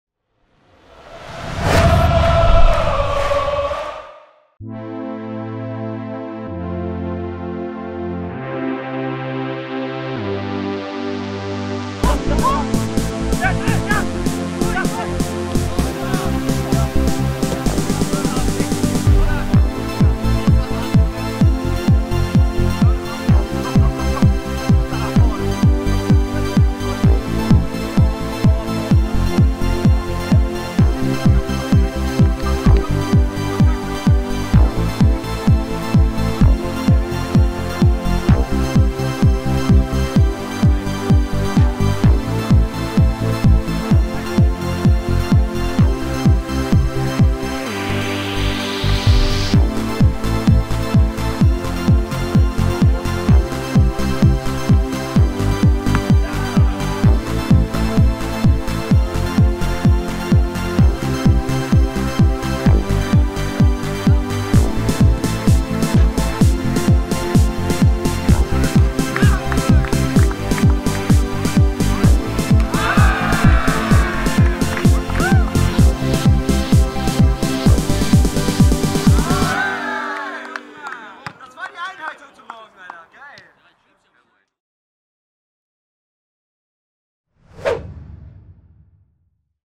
Darmstadt 98 | Zirkeltraining im Trainingslager
Am zweiten Tag des Sommertrainingscamps unterzogen sich die Lilien einem Zirkeltraining mit anschließendem Lattenschießen. Alle Eindrücke gibt's hier im Video!